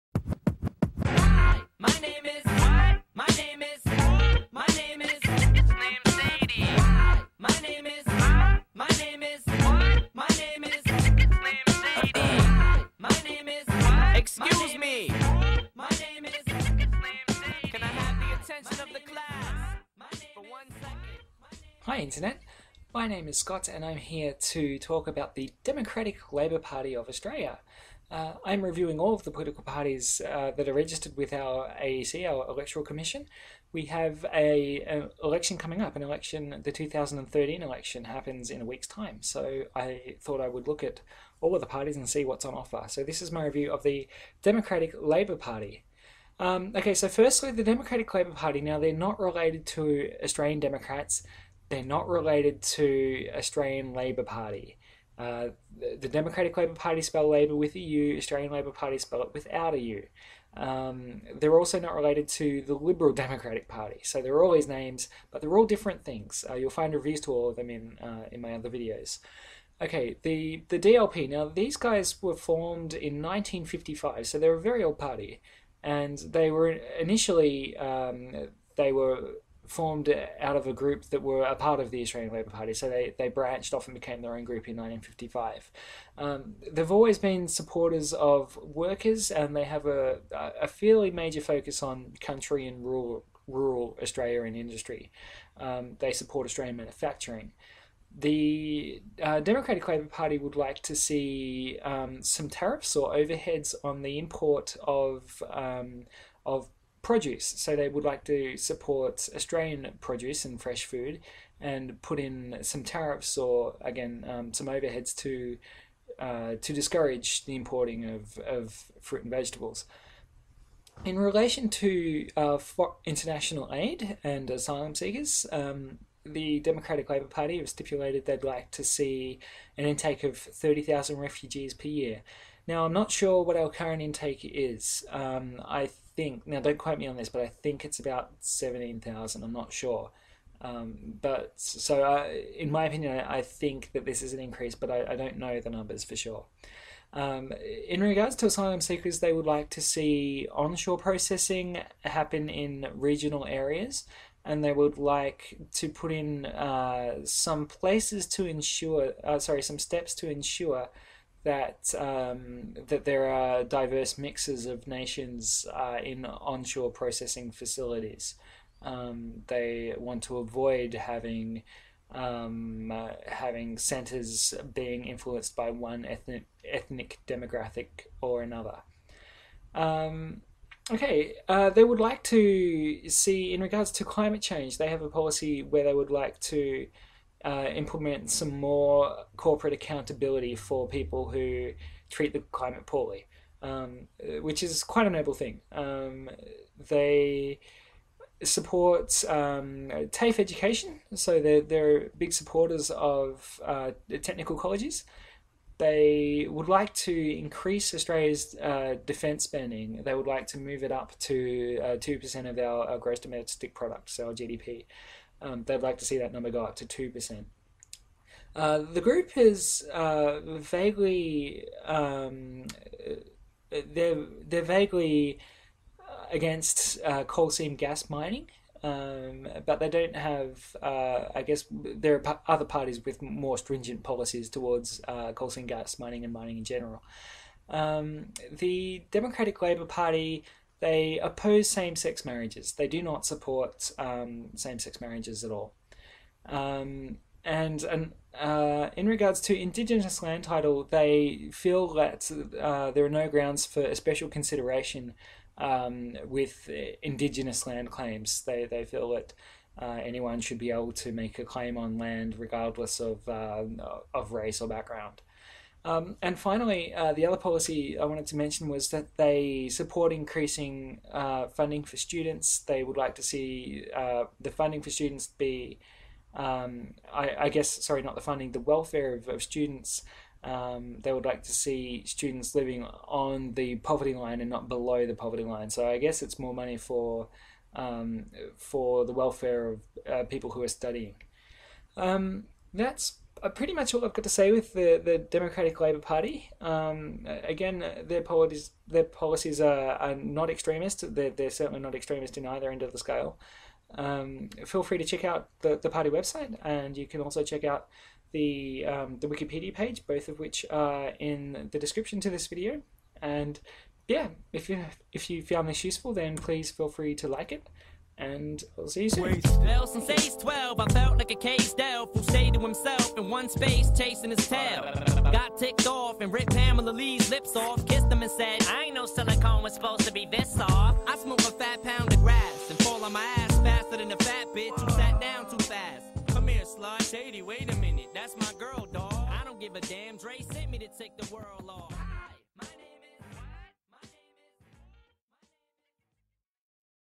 Democratic Labour Party - review of Australian political parties 2013
My review of the Australian political party, Democratic Labour Party
Shot in September 2013.